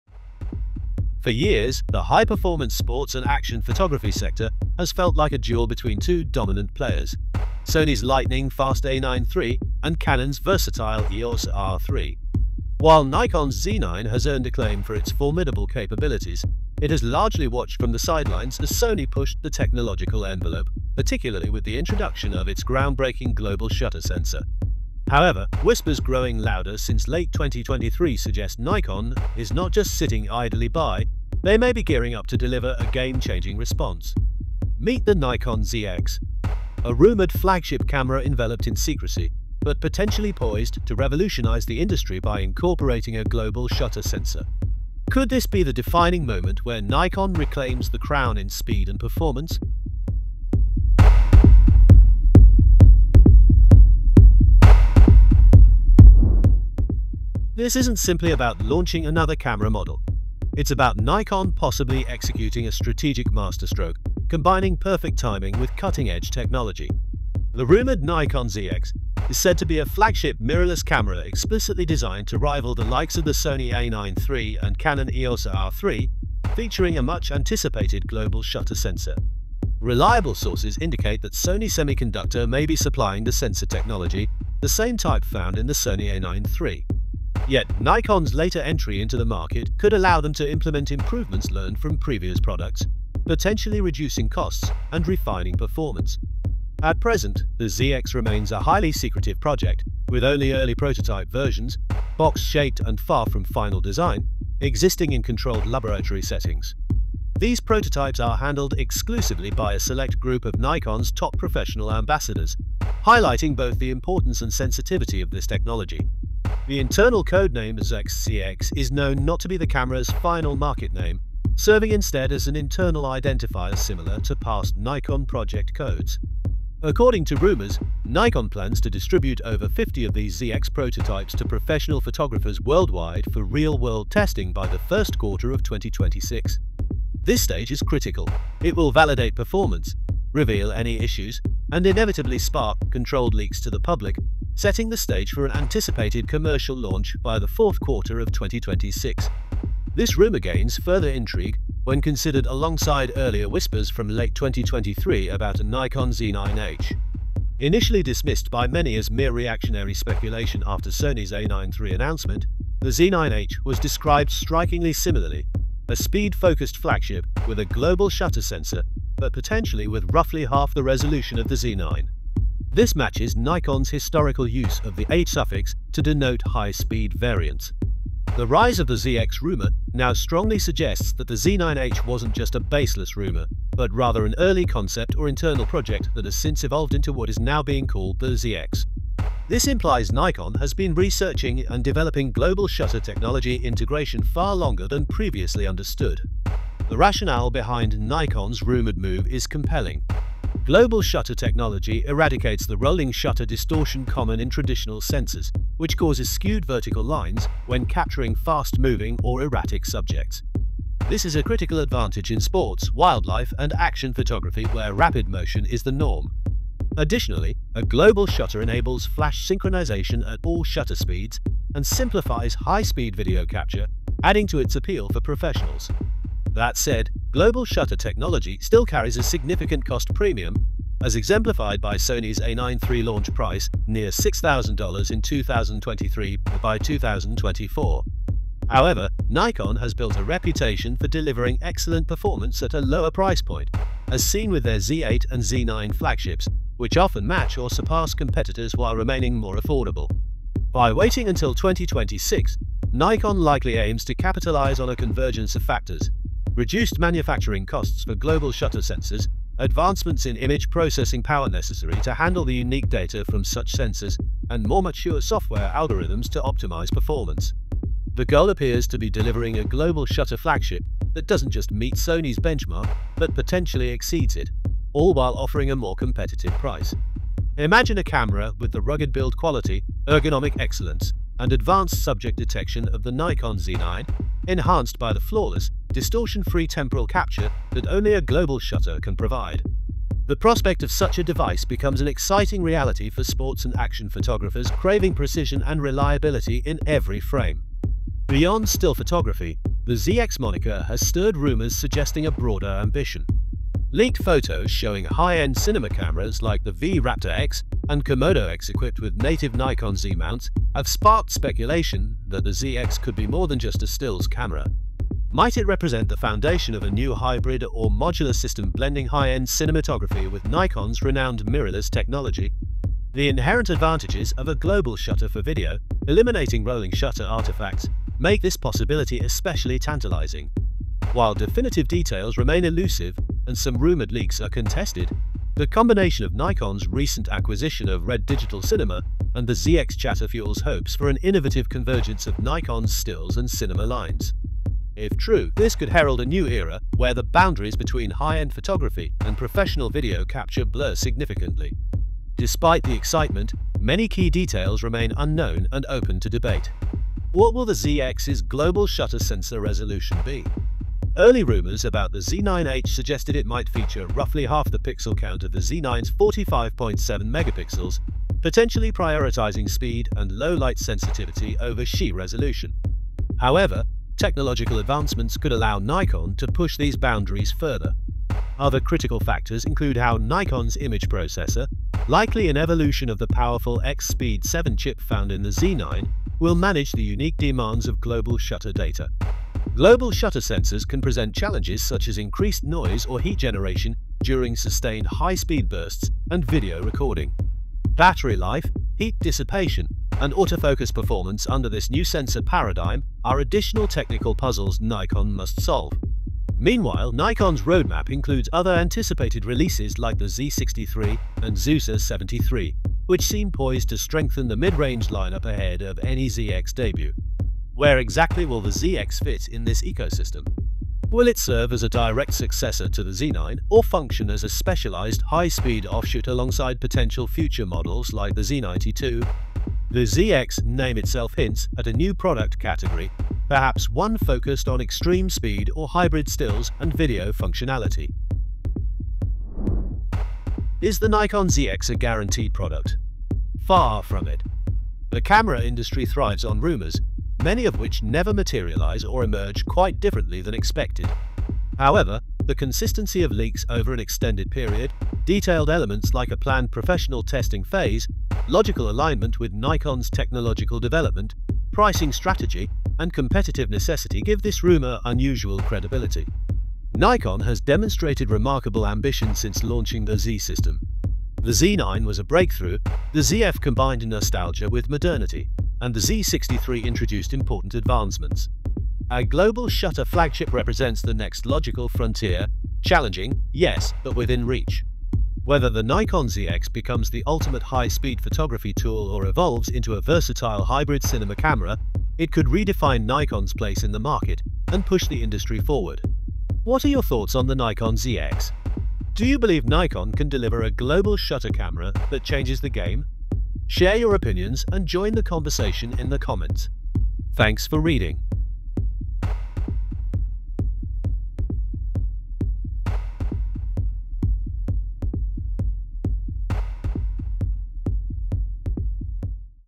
Nikon ZX - All About SENSOR!
Since late 2023, rumors have been steadily building around the Nikon ZX, hinting at a game-changing flagship camera in the works. This highly anticipated model is said to feature groundbreaking global shutter technology, setting it apart from the competition. While still cloaked in prototype secrecy, the Nikon ZX promises to deliver unmatched speed and performance. Could the Nikon ZX be the breakthrough that reestablishes Nikon’s dominance in the high-speed camera market? Fans and experts alike are eagerly watching to see if the Nikon ZX will redefine what’s possible and reclaim Nikon’s leading position.